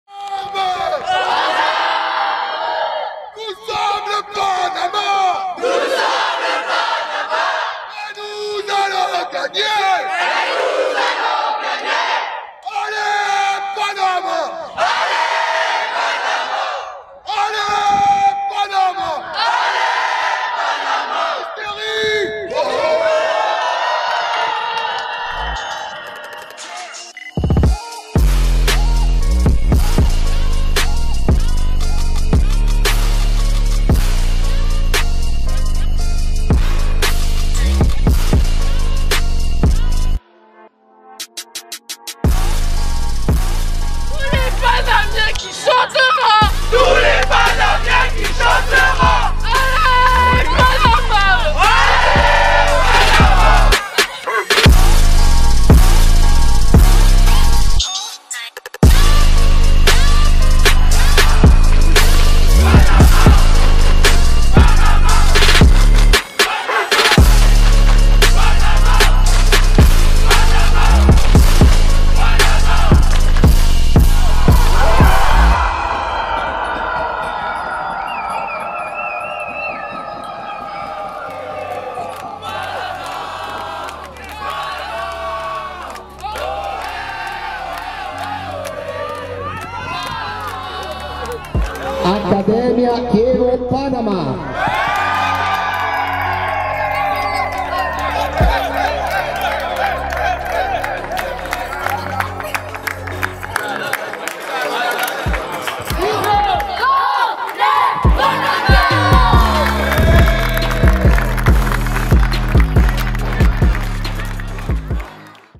Campeones! Torneo Internacional Valtenesi del Garda
Corto de los campeones U12, los 8 héroes que volvieron a dejar el nombre de Panamá en alto. Grito de Guerra que se escucha es equipo Francés que coreaba a favor de los canaleros... en el video se agradece este extraordinario gesto.

Nuestra fundación deportiva durante 15 años de desarrollo de fútbol base. Más de 600 niños han llegado europa 19 seleccionados juveniles han pasado por nuestro proyecto y dos firmas profesionales en italia, son nuestro legado al fútbol de Panamá.´